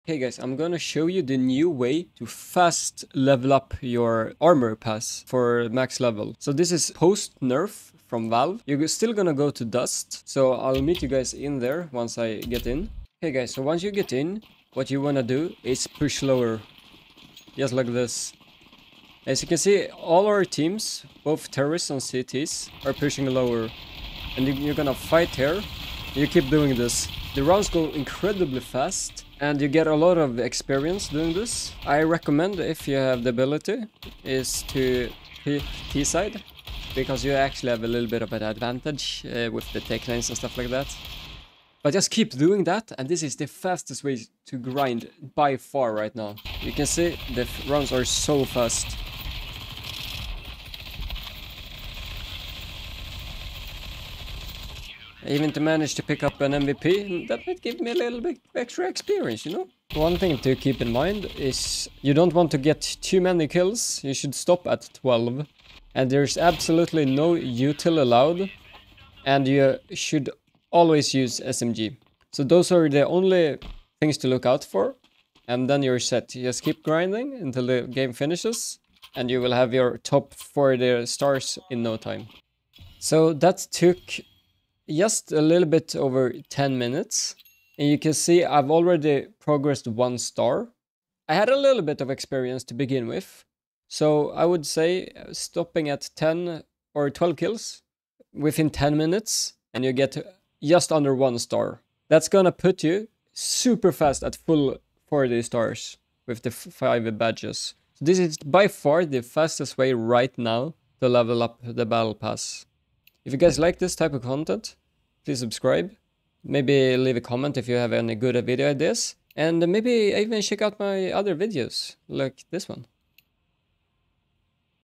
NEW fastest way to xp farm Armory pass!
Updated xp farm method post nerf for Armory pass!